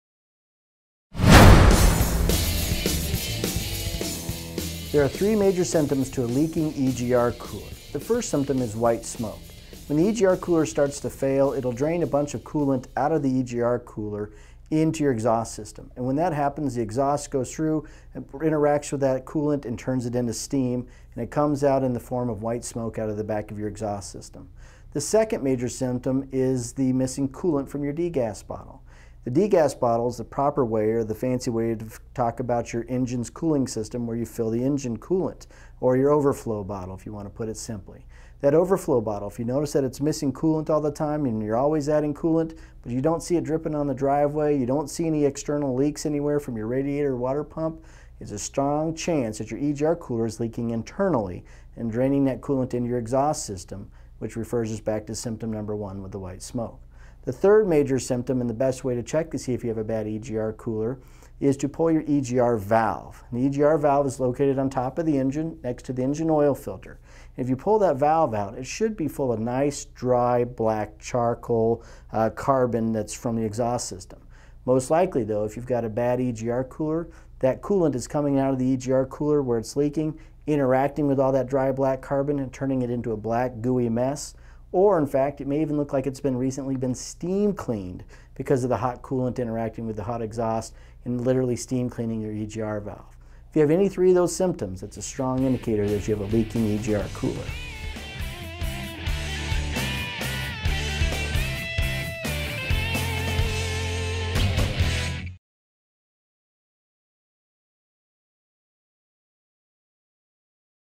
Symptoms of a bad EGR cooler
In this video, we explain the symptoms of a bad EGR cooler and valve. We do have a detailed blog post on the EGR valve